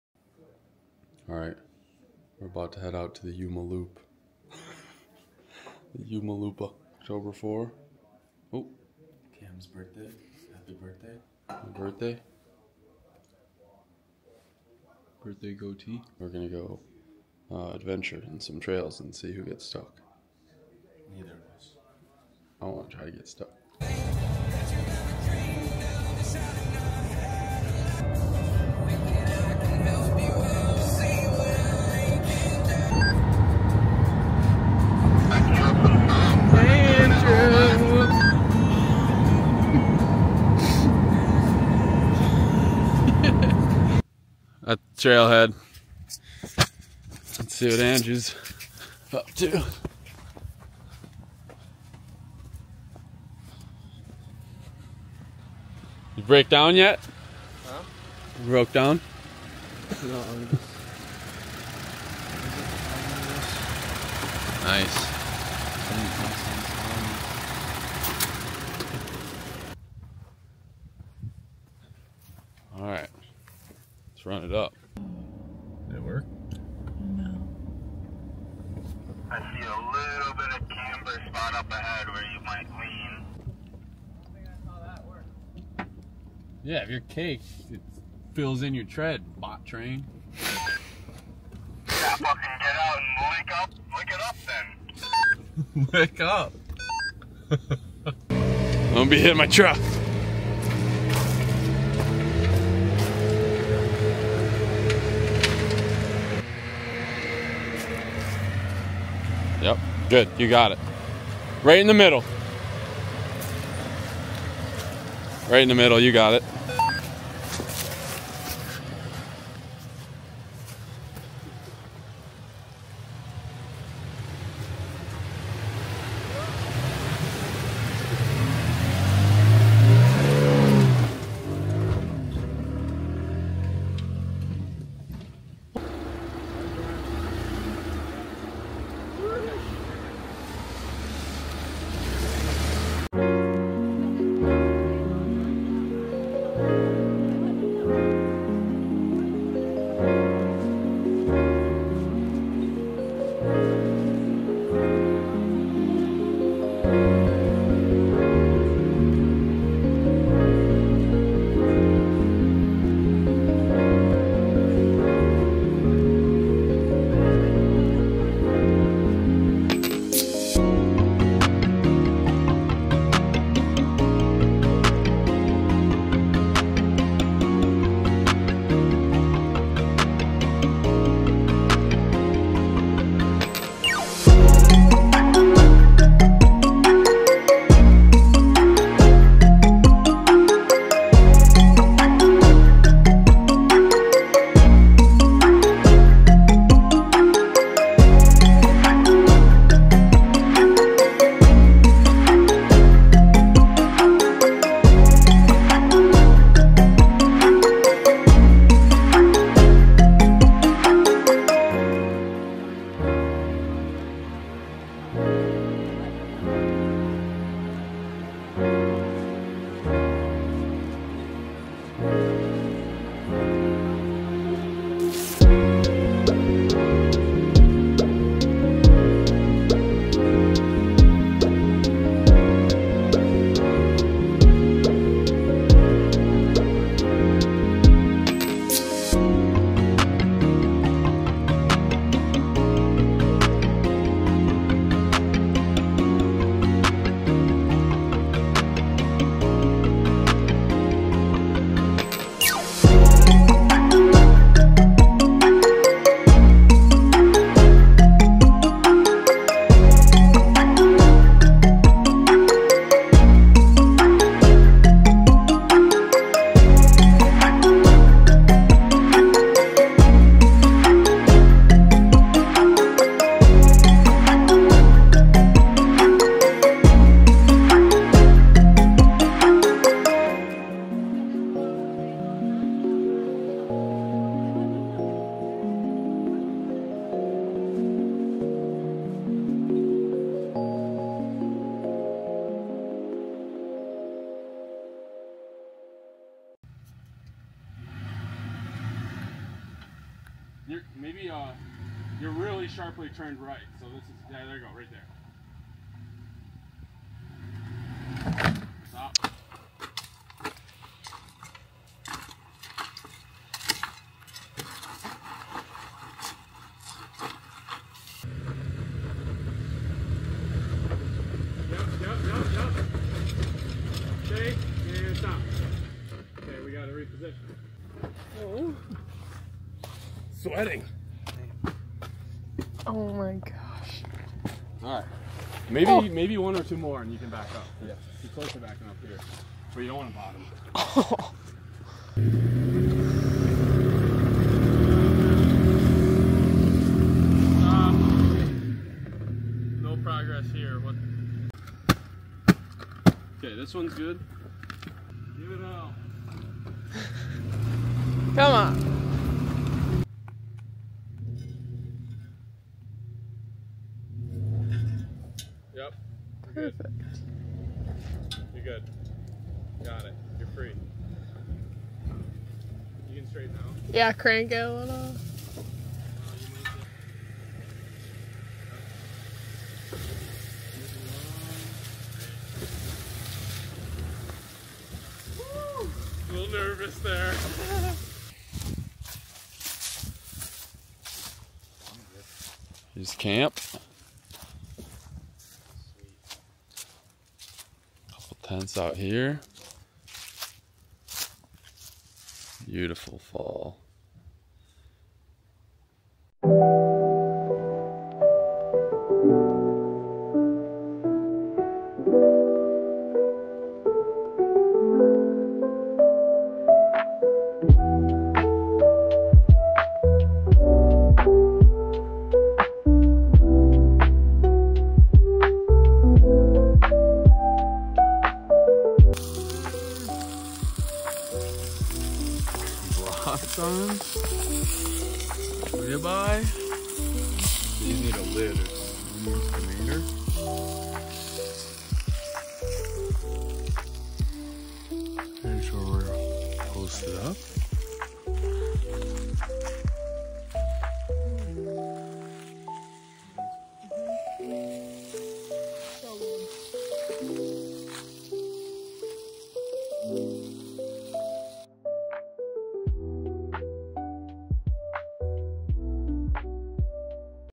Toyota Tacoma vs Ford F-150 Off-Road
Awesome trail riding / camping trip to the Yuma Hills in Michigan.